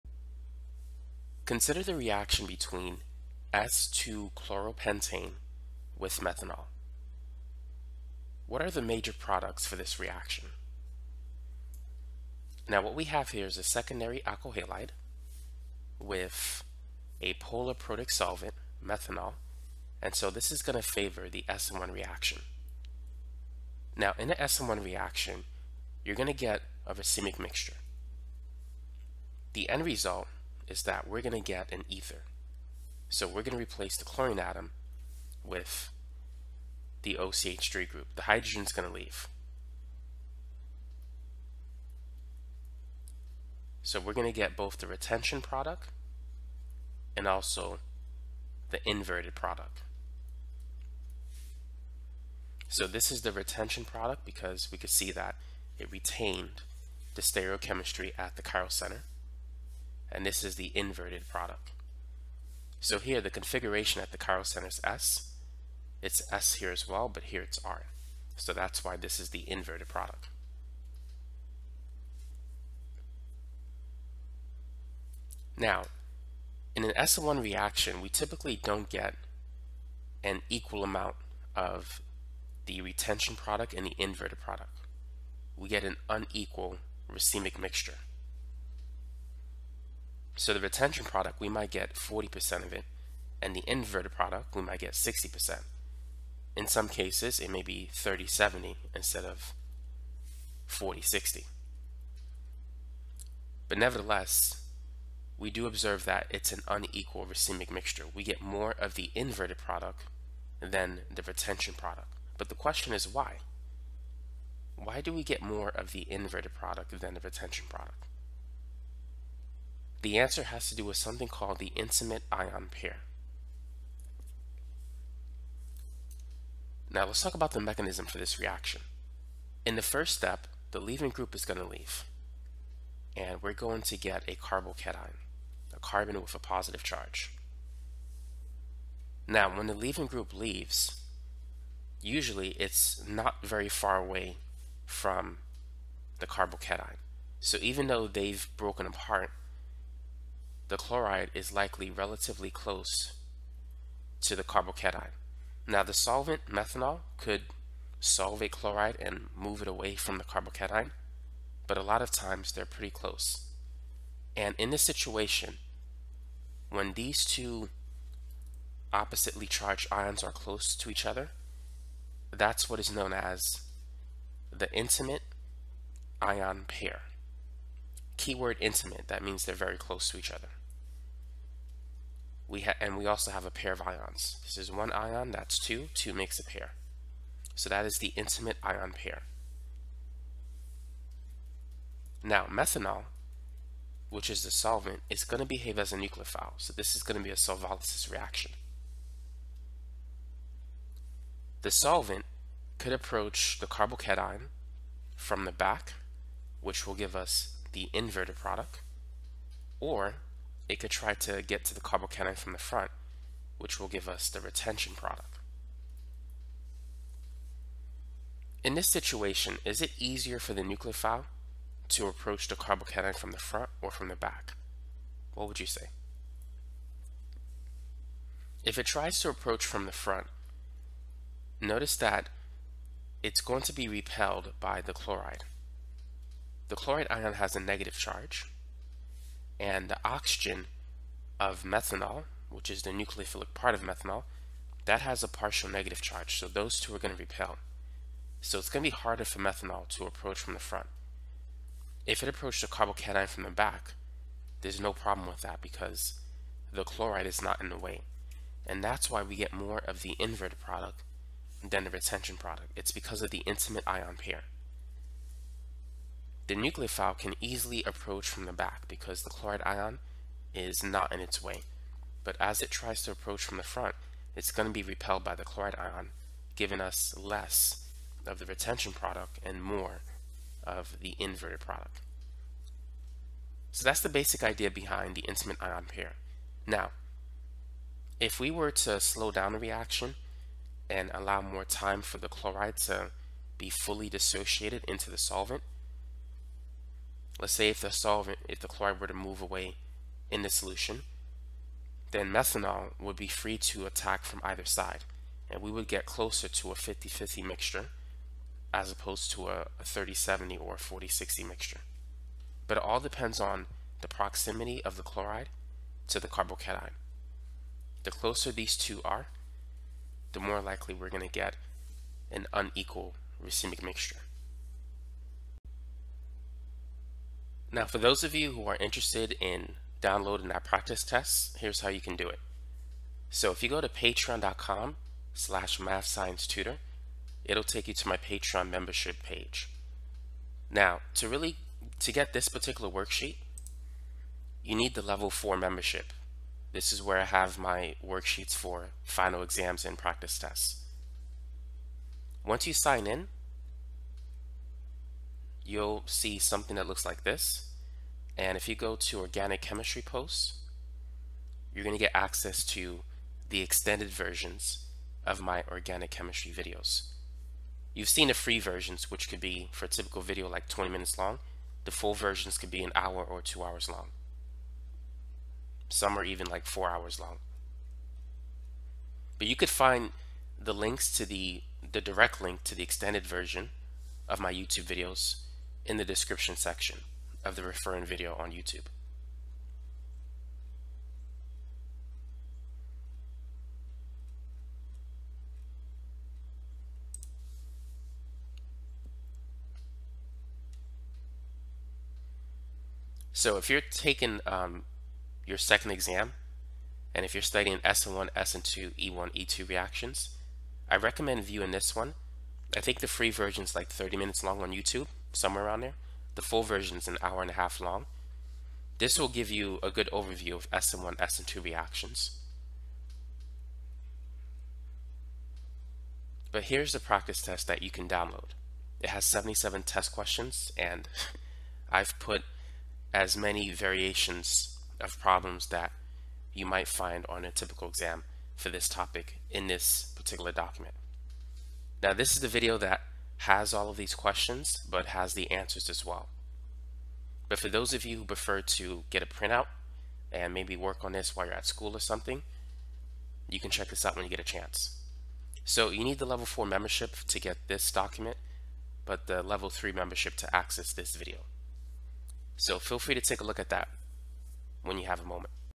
Intimate Ion Pair - Racemic Mixtures - SN1 Reaction
This organic chemistry video tutorial provides a basic introduction into the concept of the intimate ion pair and how it leads to an unequal racemic mixture in a SN1 reaction.